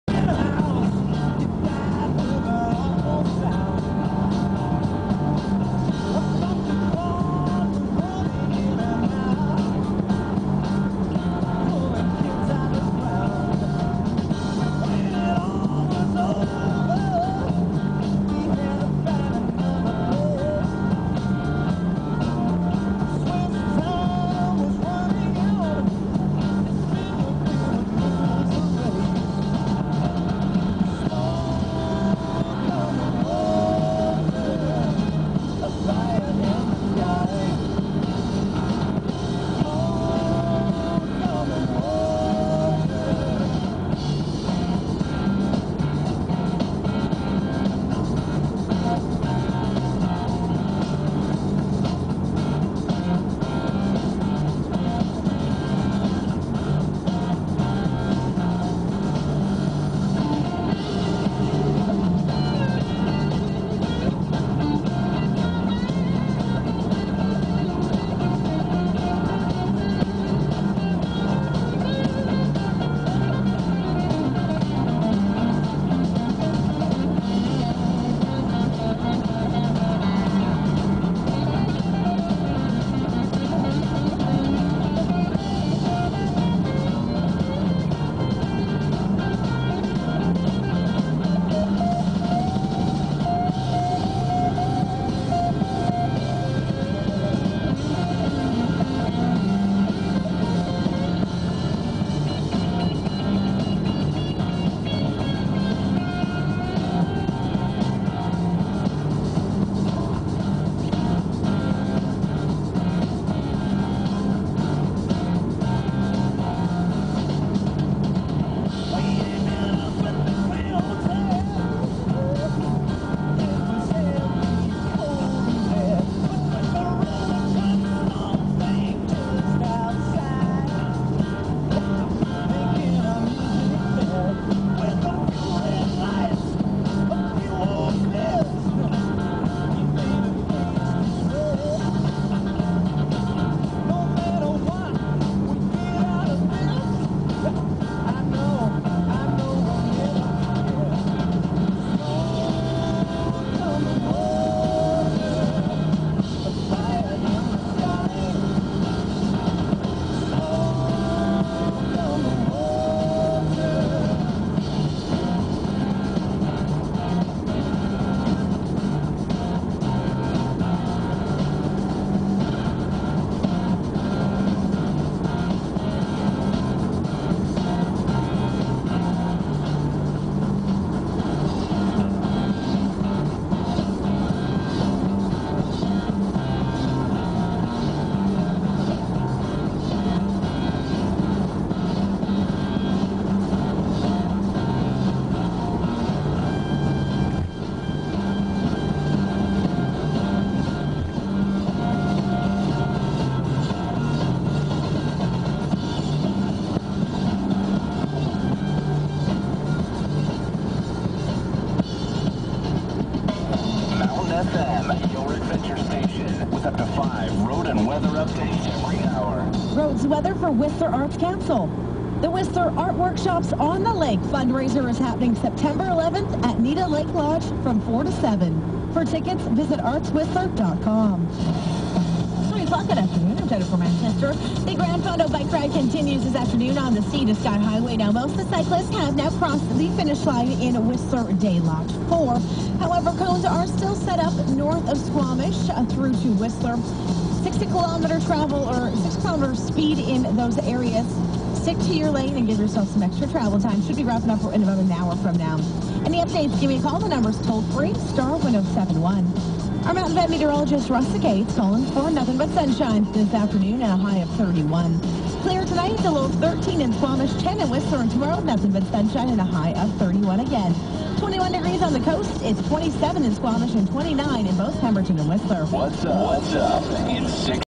Squamish _ Whistler
A drive along the highway.